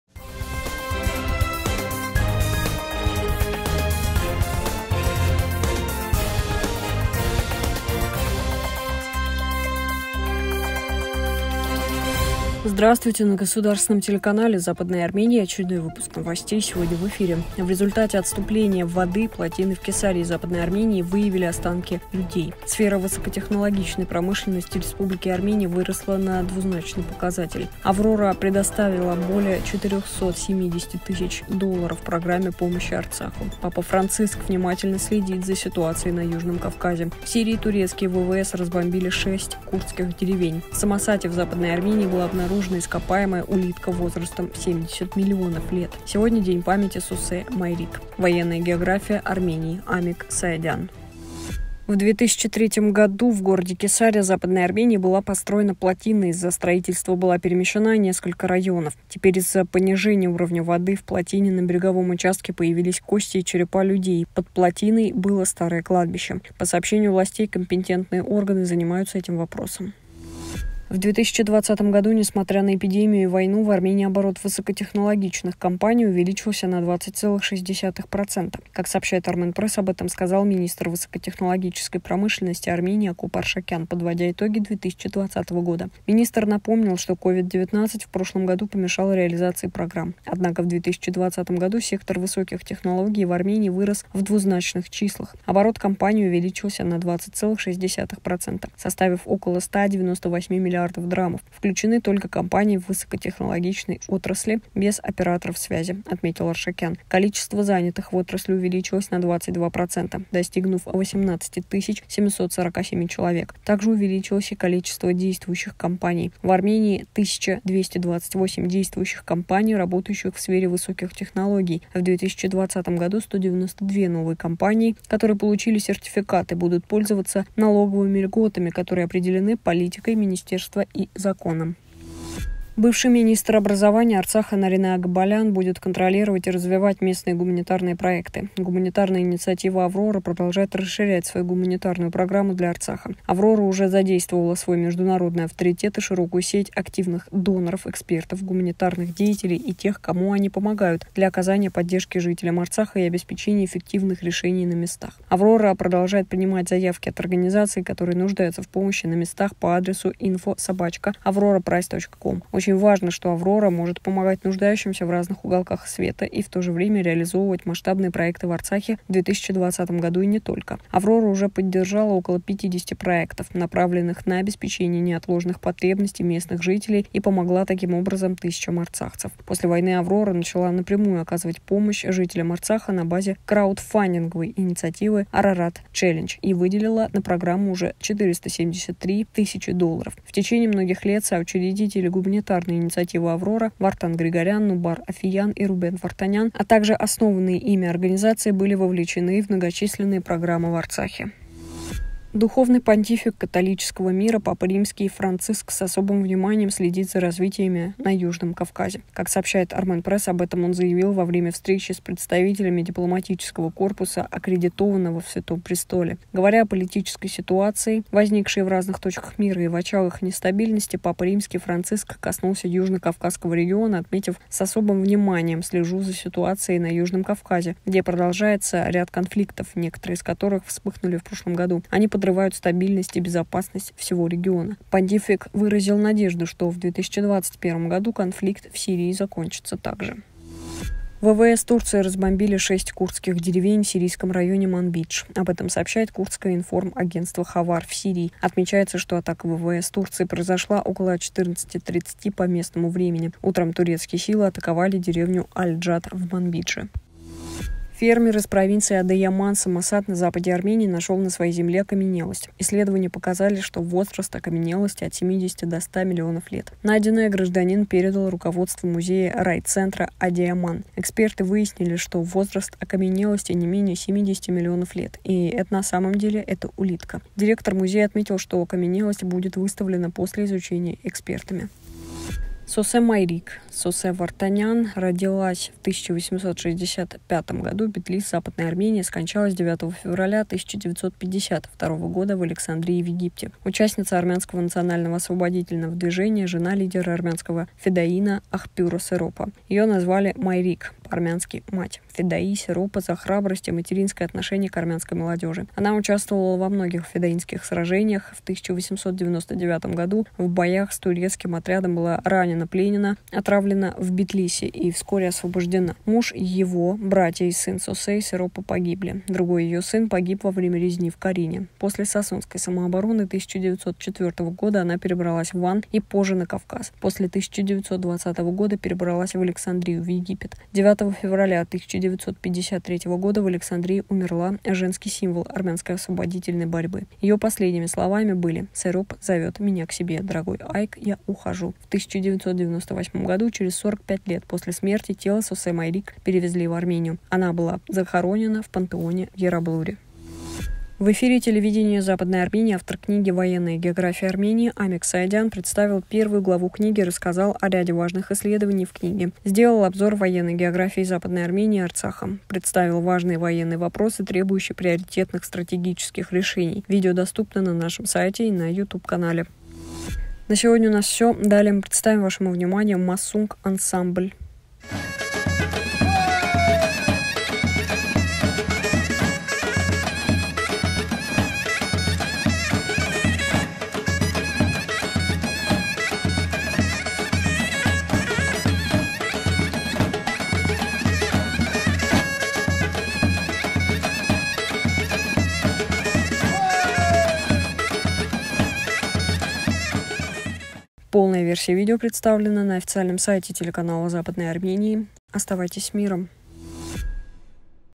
Аврора, помощь Арцаху.Новости 09.02.2021
1. В результате отступления воды плотины в Кесарии ,Западной Армении выявили останки людей
2. Сфера высокотехнологичной промышленности Республики Армения выросла на двузначный показатель.
3. Аврора предоставила  более $ 470 000 программе помощи Арцаху
4. Папа Франциск внимательно следит за ситуацией на Южном Кавказе.
5. В Сирии турецкие ВВС разбомбили 6 курдских деревень.
6. В Самосате, Западная Армения, была обнаружена ископаемая улитка возрастом 70 миллионов лет.
7. Сегодня День памяти Сосе Майриг.
8. «Военная география Армении» ․ Амик Саядян